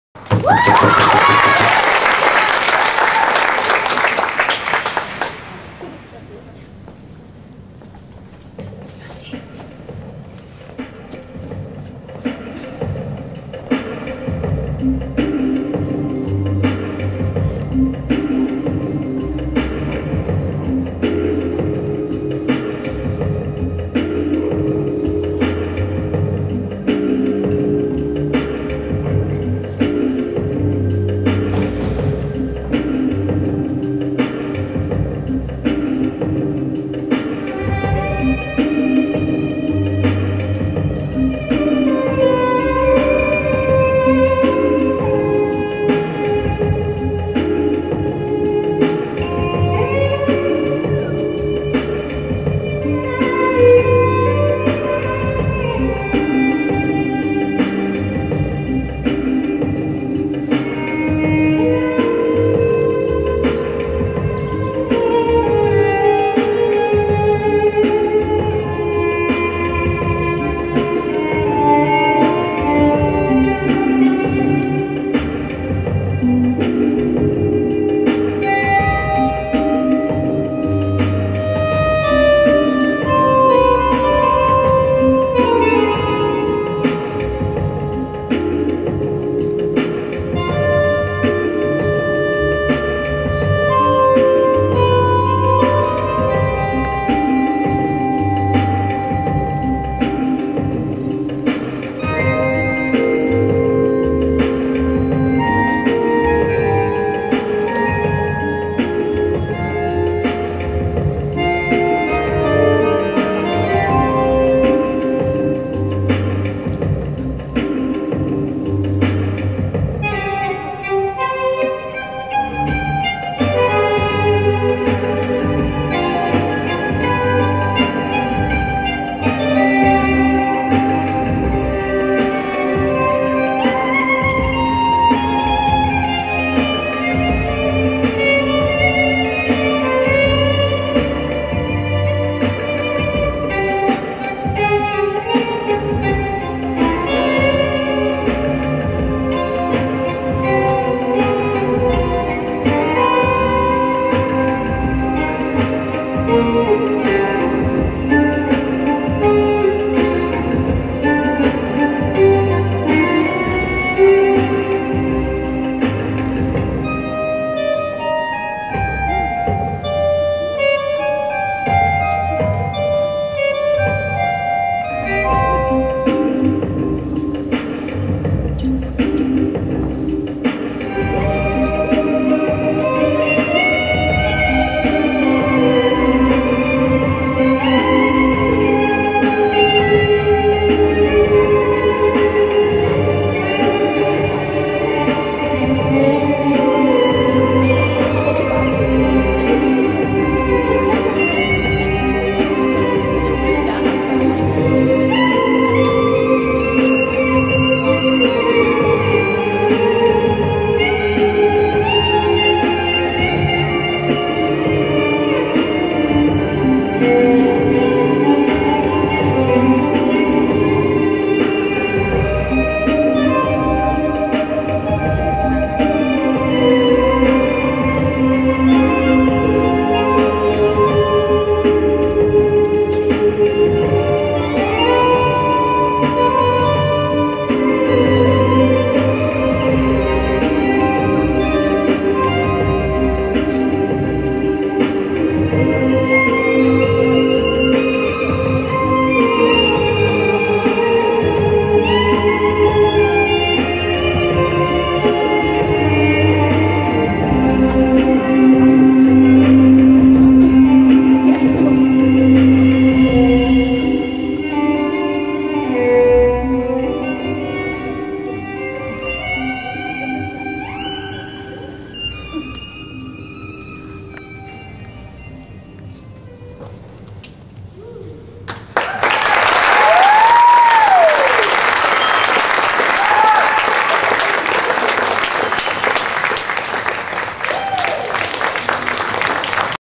Tango Nuevo Demo. Auckland University of Technology
Kasha Szot and Grant Fromont demonstrate Tango Nuevo at the AUT School of Dance